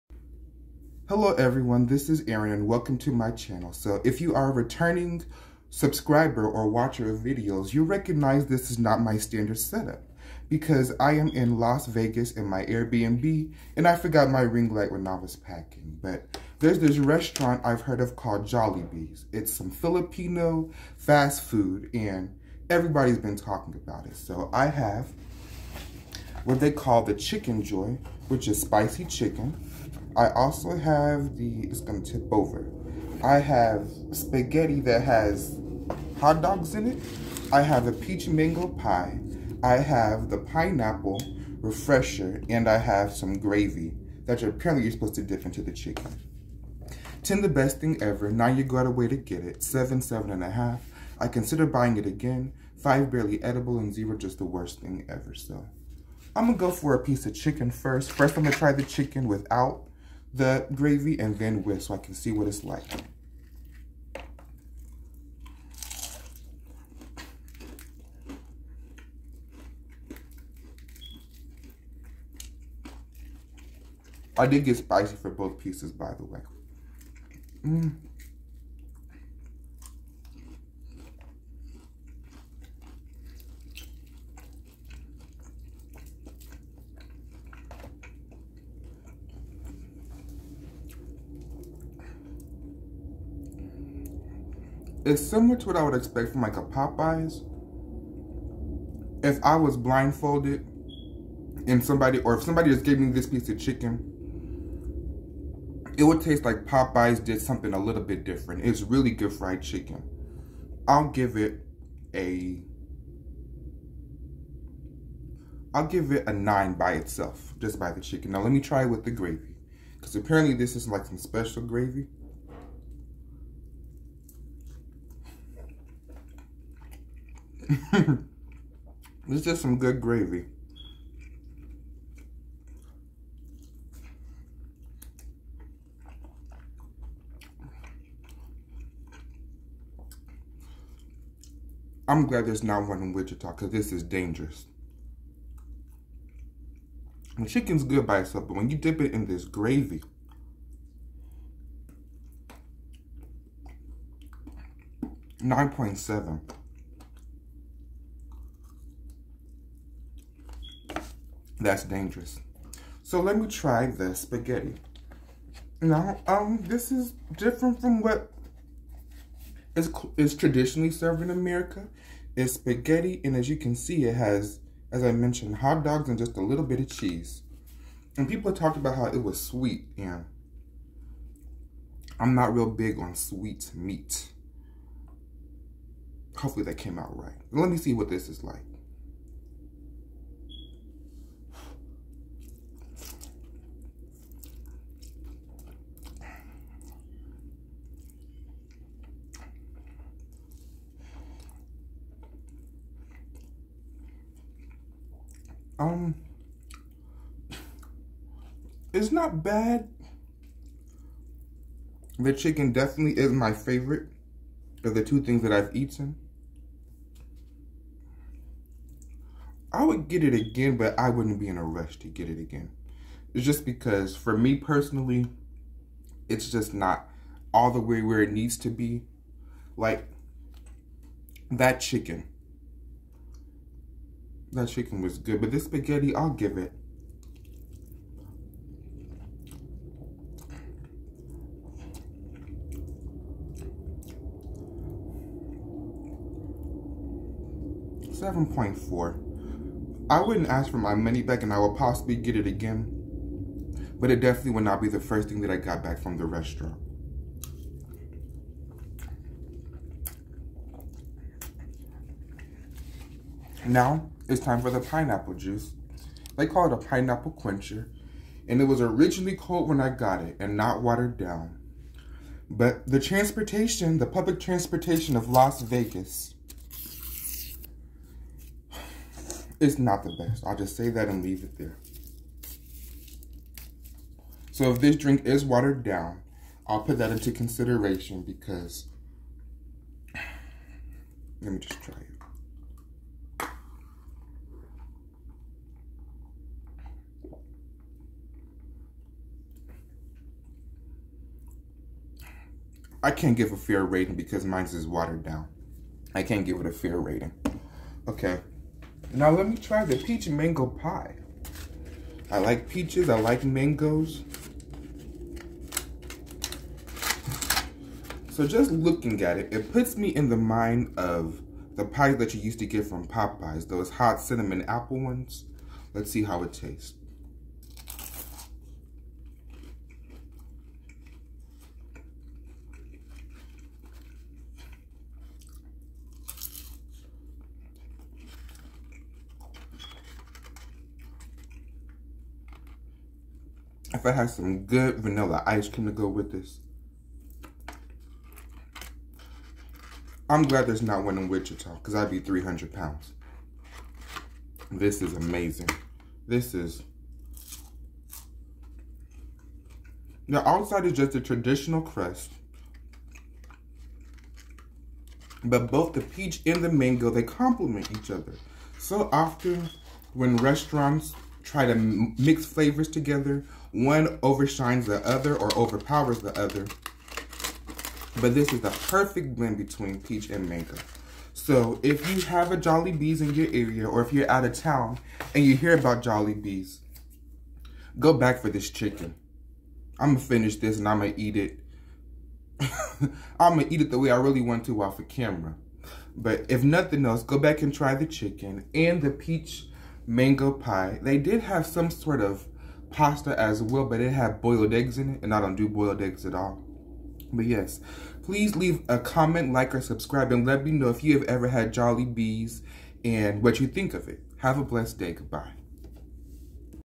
JOLLIBEE taste test! (Chicken joy, spaghetti & peach mango pie)
I forgot to mention, the peach mango was a 9.7! It's definitely a place worth trying.